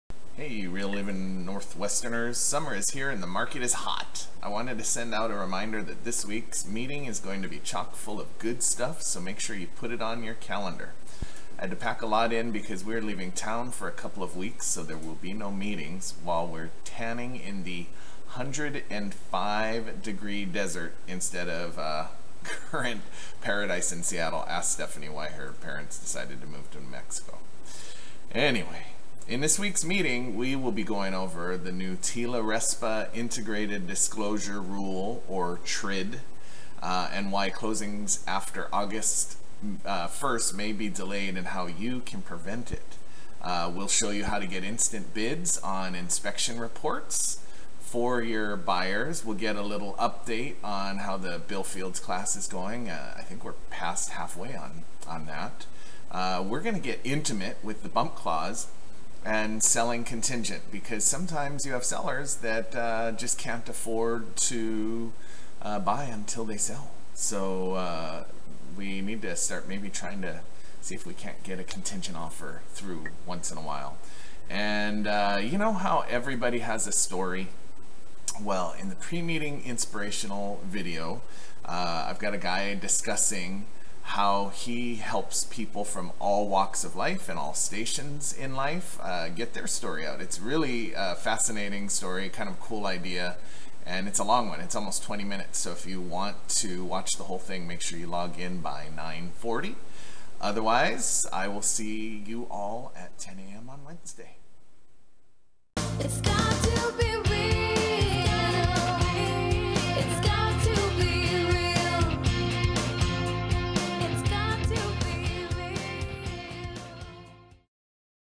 Keoki's Weekly Reminder
The following is an excerpt from our Weekly Online Meeting. In this clip Keoki McCarthy reminds us about the upcoming weekly meeting and all the juicy stuff it entails as well as upcoming events.